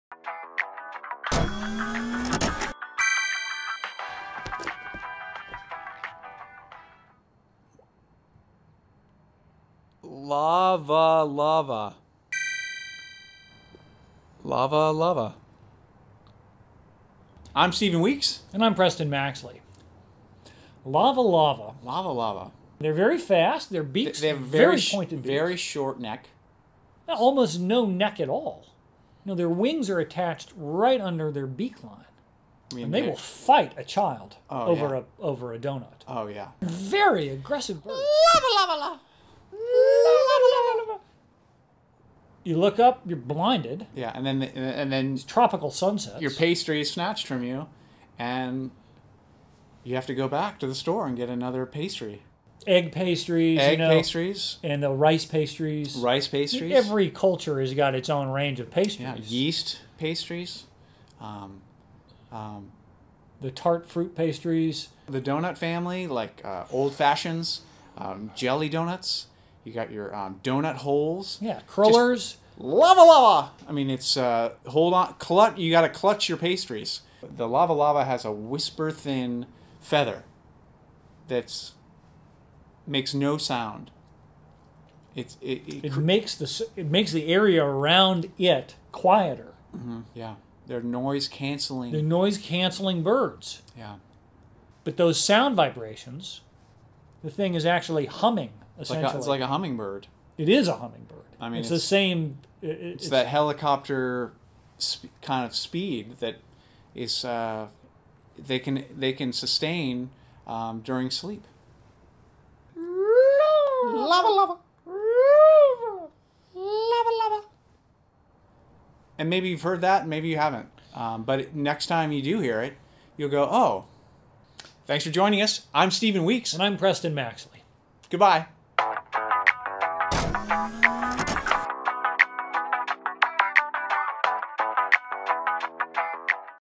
This Weeks' Word Week 5
The fifth installment in this ongoing series which seeks to share an important Life Studies Institute tradition of discussion and inquiry.  With Dr. Stephen Weeks, and featuring faculty guest Preston Maxley.